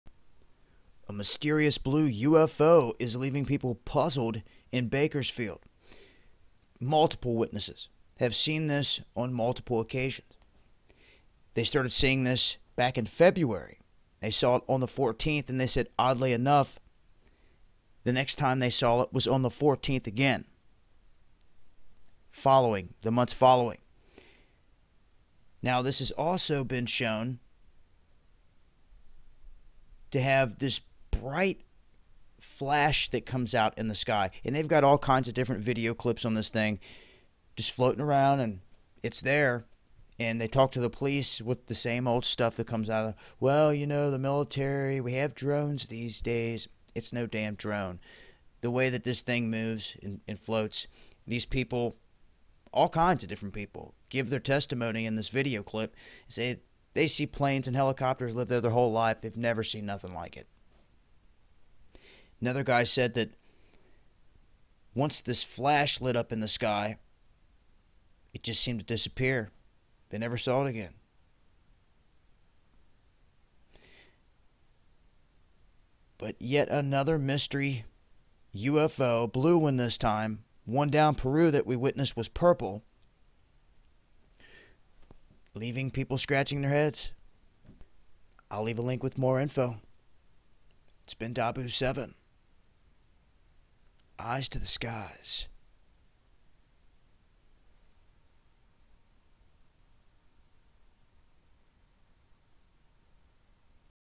Mysterious Blue UFO Leaves People In Bakersfield Puzzled
Several neighbors living off Alderpointe Street near Panama and Gosford in southwest Bakersfield  say they first witnessed a mysterious blue light in the sky back in February, and again in March. Both times they saw it on the 14th.

"I was taking the recycling out to the trash can, and I saw this blue light in the sky, and I thought, that's kinda bright to be a star, I've never seen a blue star be that bright," said Kelly Castruita.

"Watched it for about 10 minutes, just doing weird stuff, it would turn on and off and it would disappear," added Kelly's husband Jeff.

Neighbors said the blue light was about the size of a dime, and started off just above the skyline, and then migrated northwest.

"It pretty much flew, stopped, then started doing some upward motion and downward motion, so I've never seen anything like it," said neighbor Randy Gabriel.